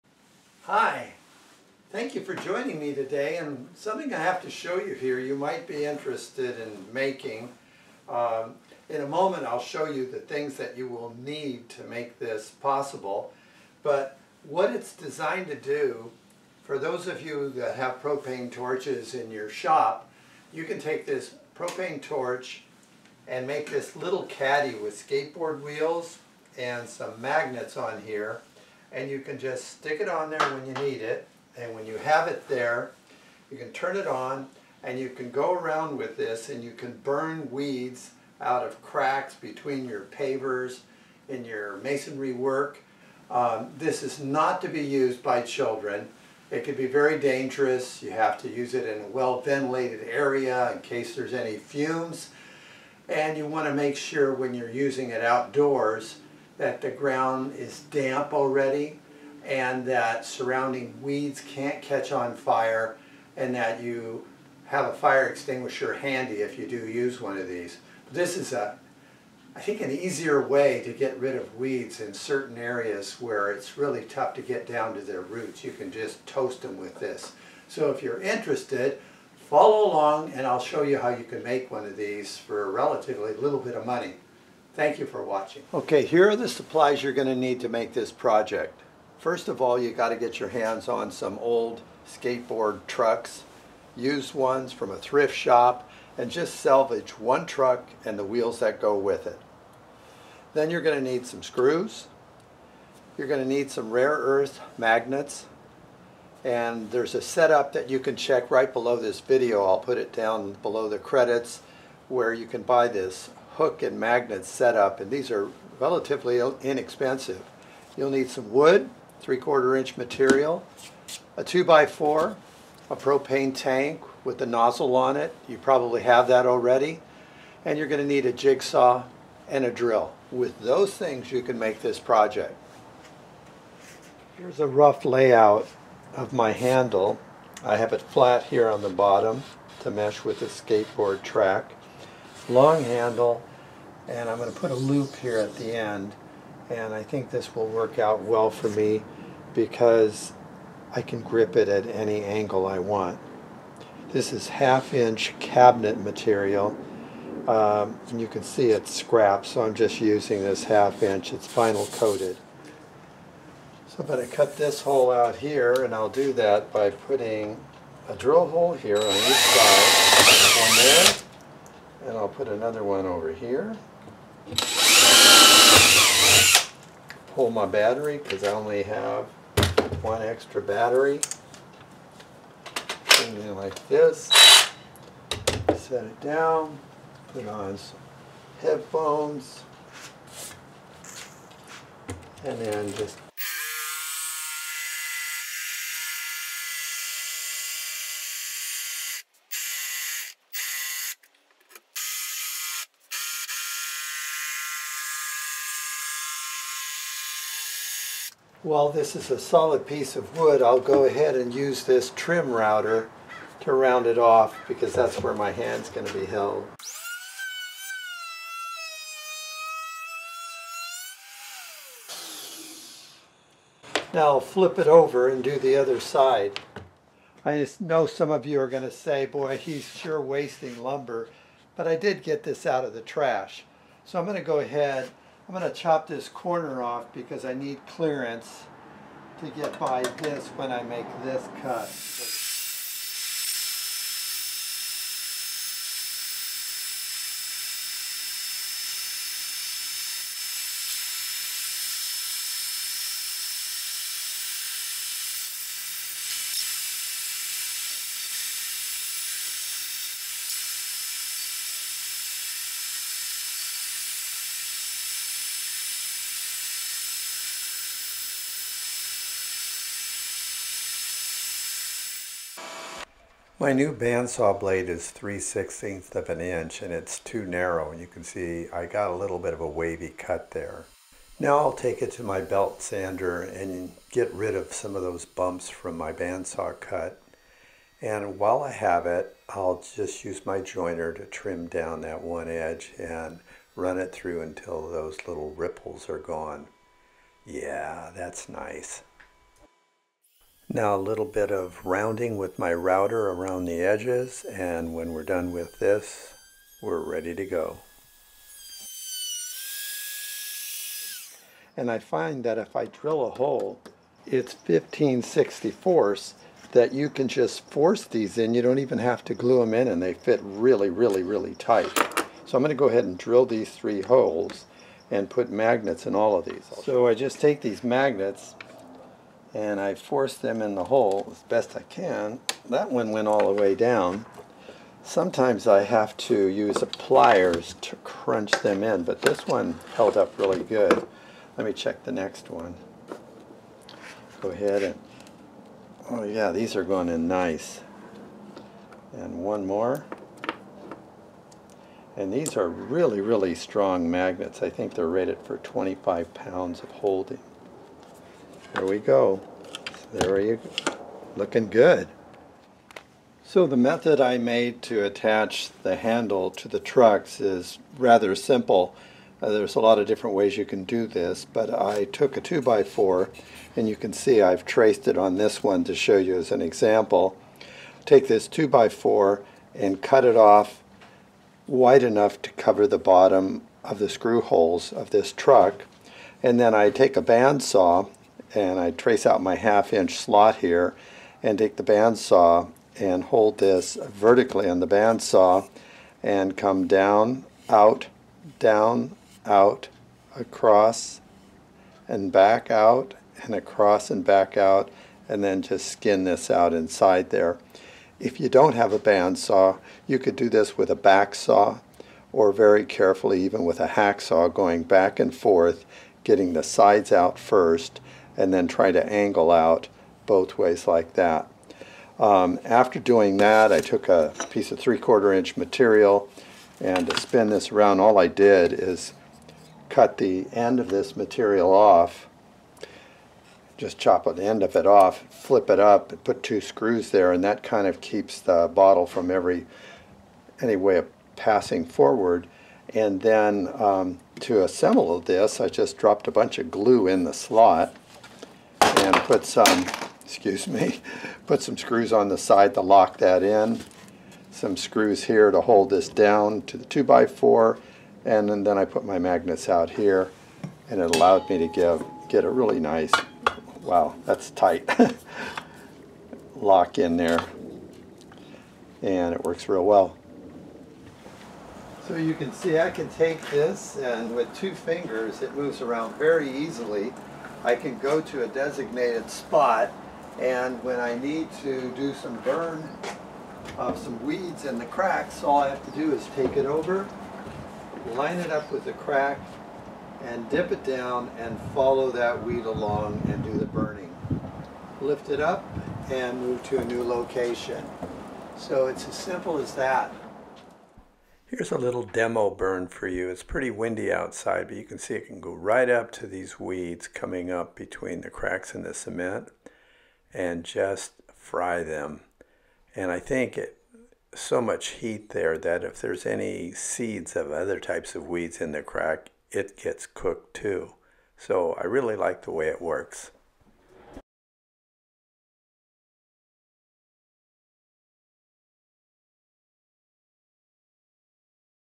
DIY Weed Burner
Simple steps are given for the construction of a propane "weed burner".  The burner works excellently, especially with weed growth between pavers and other masonry areas.
Magnets @ 53 Cents each: